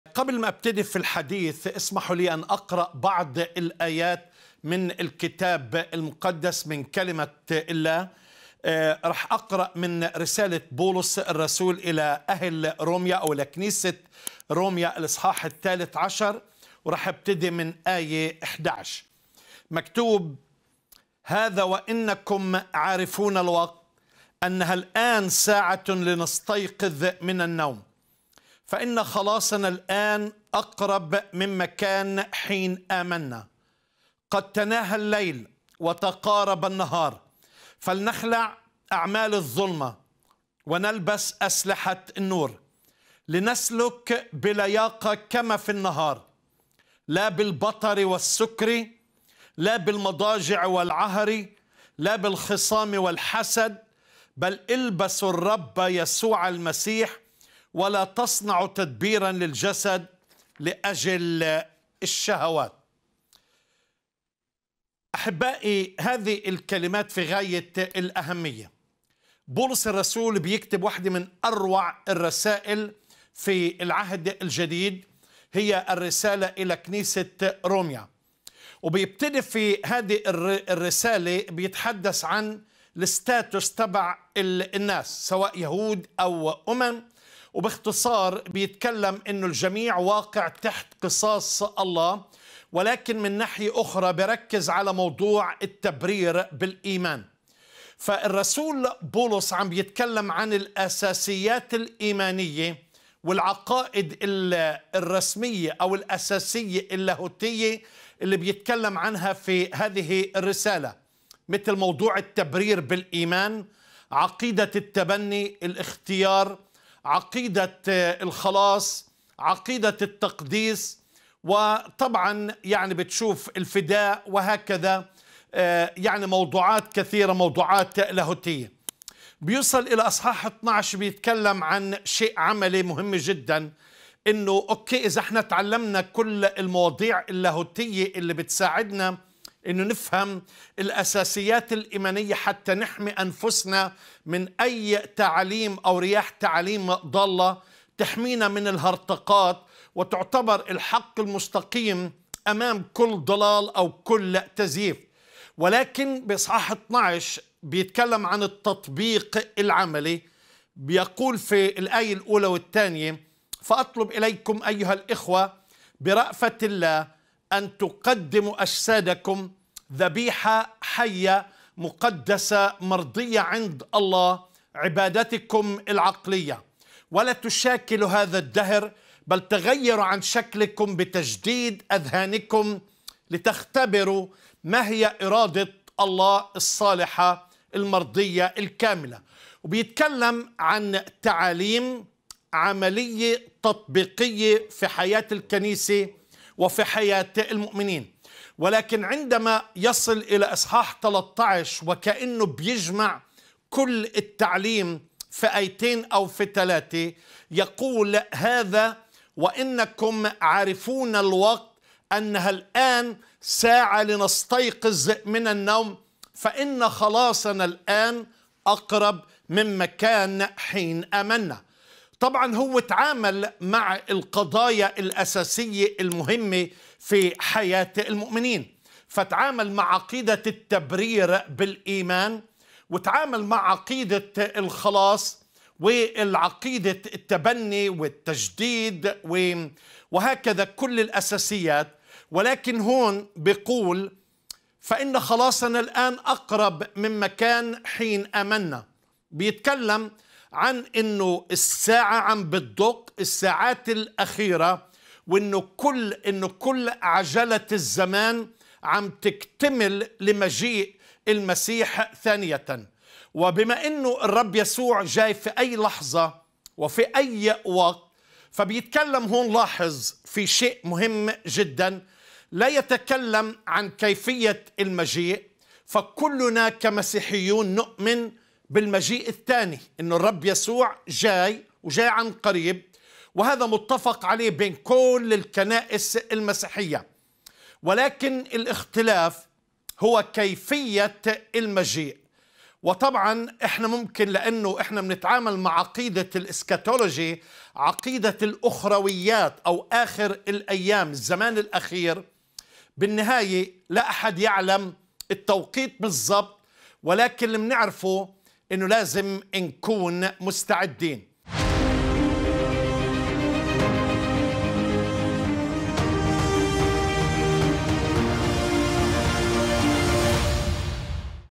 الساعات الأخيرة تعلن عن قرب مجيء المسيح ثانية
📌 هل نحن مستعدون لمجيء المسيح؟

🟩 خطوط عريضة تلخص الموضوع:
🔸 دعوة للاستيقاظ الروحي والاستعداد لمجيء المسيح.
🔸 التركيز على حياة القداسة وترك أعمال الظلمة.
🔸 التأكيد على عقائد أساسية: التبرير بالإيمان، الخلاص، التقديس.
🔸 أهمية التجديد الداخلي والتطبيق العملي للإيمان في حياة الكنيسة والمؤمنين.
🔸 الدعوة لفهم الزمن الحالي كوقت للعودة إلى الله واستعداد للحياة الأبدية.

❓كيف يمكننا الاستعداد الروحي في حياتنا اليومية لاستقبال المجيء الثاني للمسيح؟

📌يمكنك التواصل معنا بصورة شخصية عن طريق رسائل الواتساب👇
WhatsApp الواتساب:    🆆🅷🅰🆃🆂🅰🅿🅿